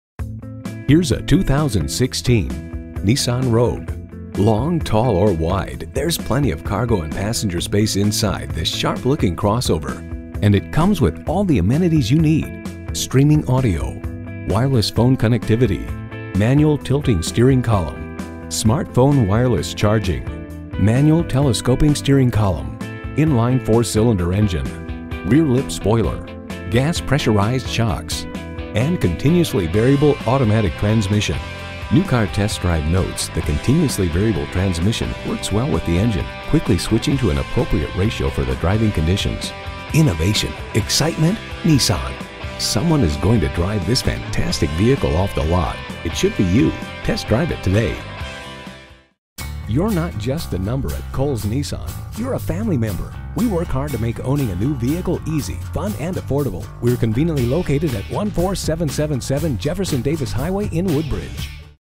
Used 2016 Nissan Rogue Washington DC VA Woodbridge, VA P14964 - SOLD
Year: 2016
Make: Nissan
Model: Rogue
Trim: S
Engine: 2.5L I4 DOHC 16V
Transmission: CVT with Xtronic
Color: Gray
Interior: charcoal
Mileage: 32263
Stock #: P14964
VIN: KNMAT2MV6GP612735

Address:
14777 Jefferson David Hwy